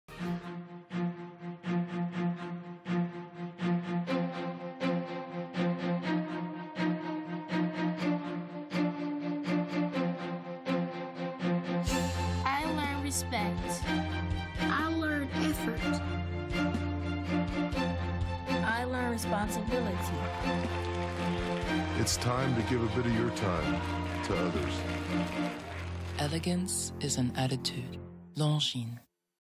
Longines - Agassi Graf
808 was invited on set to media manage, pre-edit, provide VTR playback and client Video village. Thanks for having us!